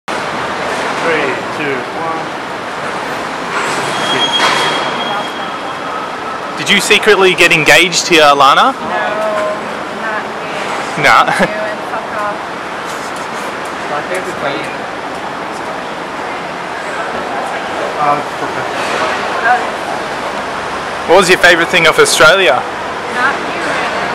Lana del rey tells Paparazzi to Fu*k off when questioned about ring
Lana del rey tells Paparazzi to Fu*k off when questioned about ring she had worn in sydney on her wedding finger